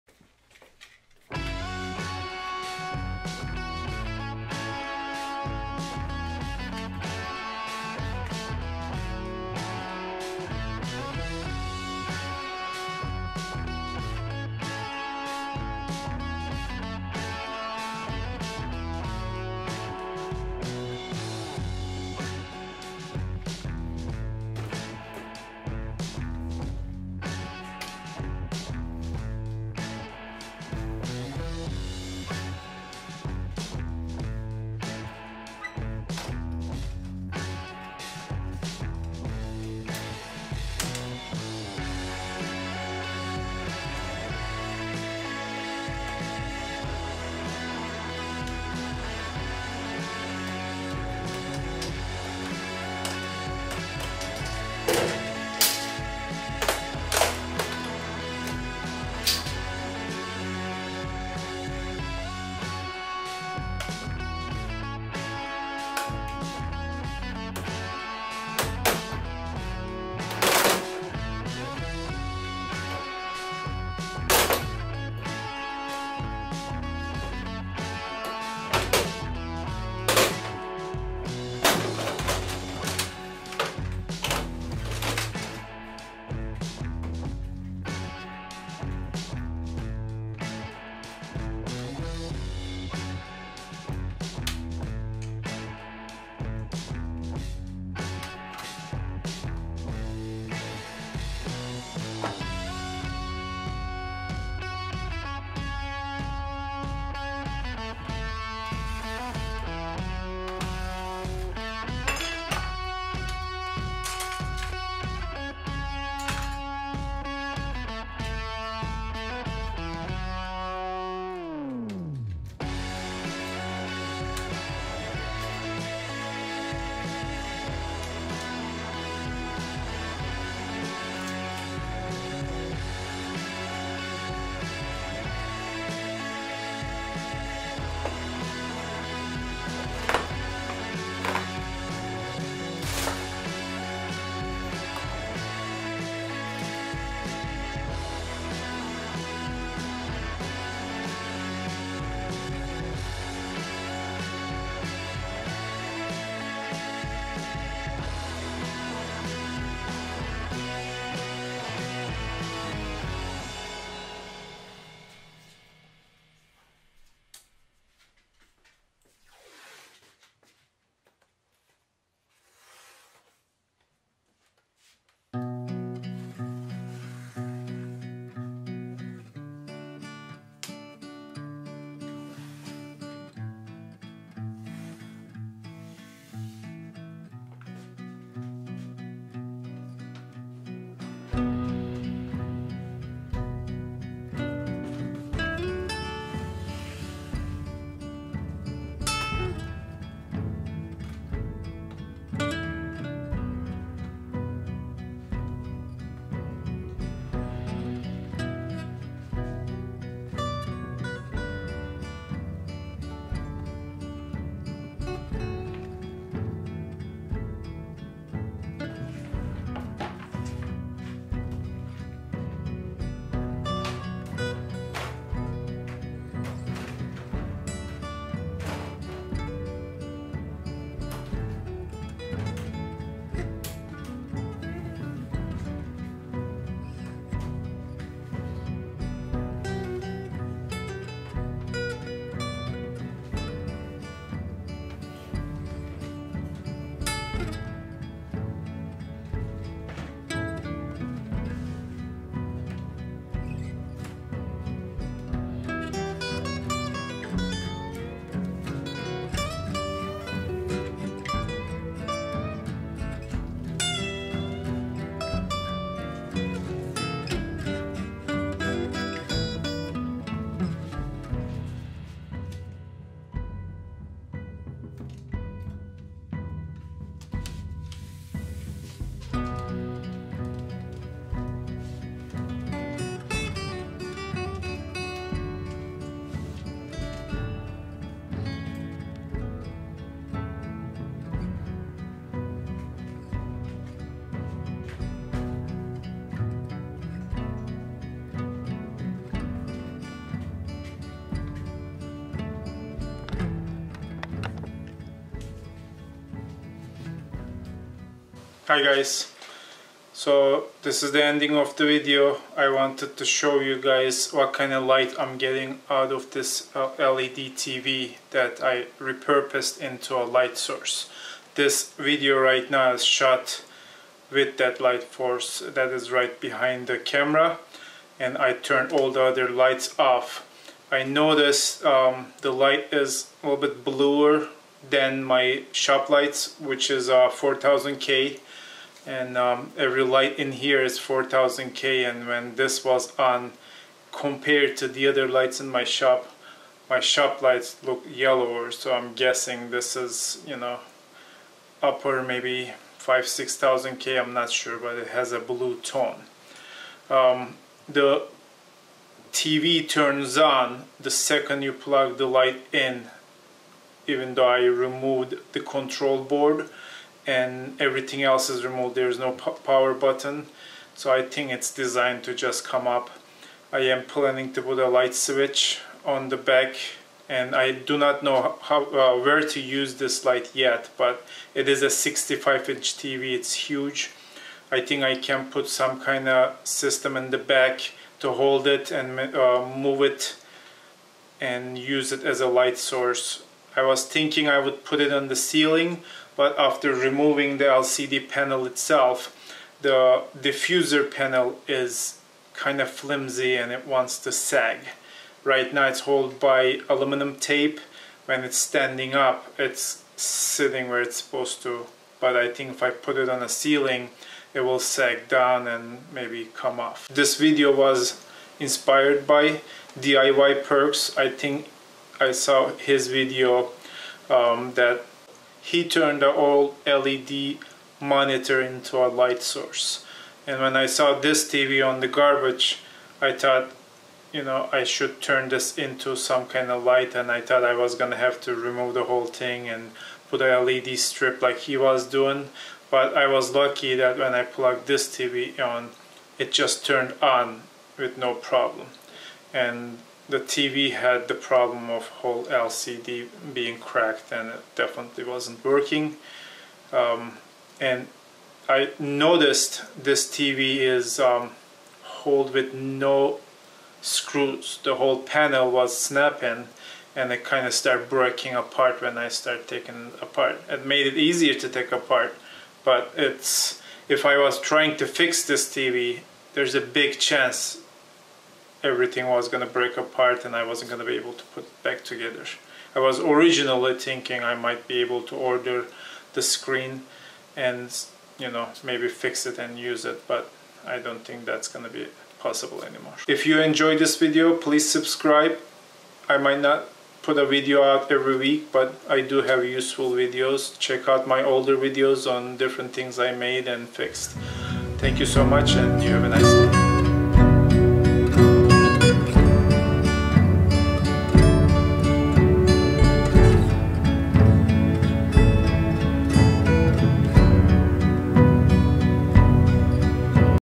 I tuned broken 4K TV into LED Light Panel in 15 Minutes
I was given this Samsung 65" UHD Smart TV that was found on a Chicago alley. It had a broken LCD panel(part that gives the picture).
There was no screws holding the outer shell. I removed the trim and broke most of them while doing this. Removed the speakers and the main board(again, no screws) . I was surprised to find, Power unit coming right up when it was plugged to the wall with the main board disconnected.  Removed the broken LCD panel and the cables to it. Put the back cover to cover up the power supply and hold everything together. Taped the sides with aluminum tape (Found on HVAC department on hardware store ) .